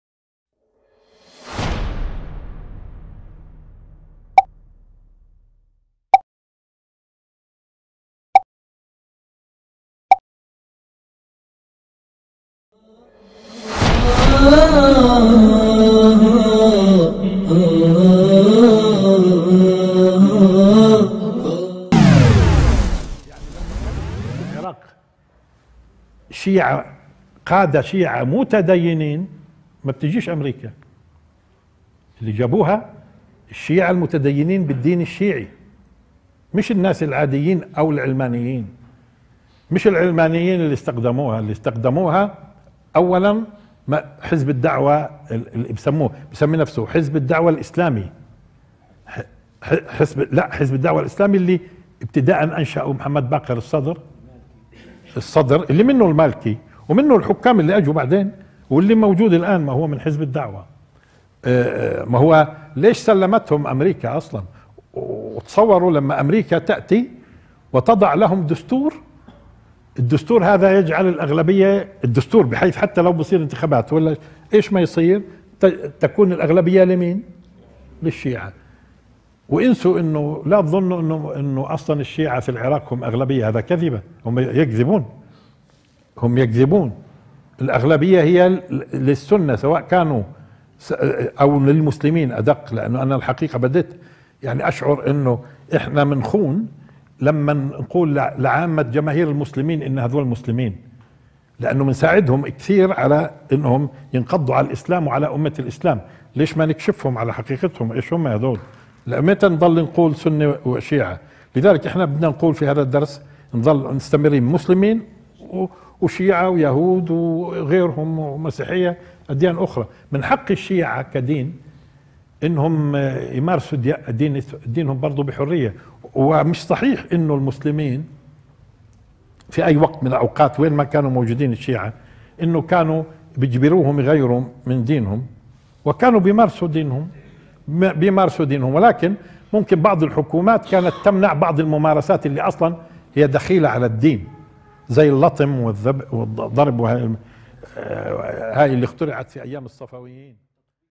الشيخ بسام جرار | لو كان هؤلاء فى العراق لما احتلتها امريكا | Sheikh Bassam Jarrar
الشيخ بسام جرار يقدم سلسلة محاضرات حول تفسير القرءان الكريم والاعجاز العددي فى القرءان الكريم .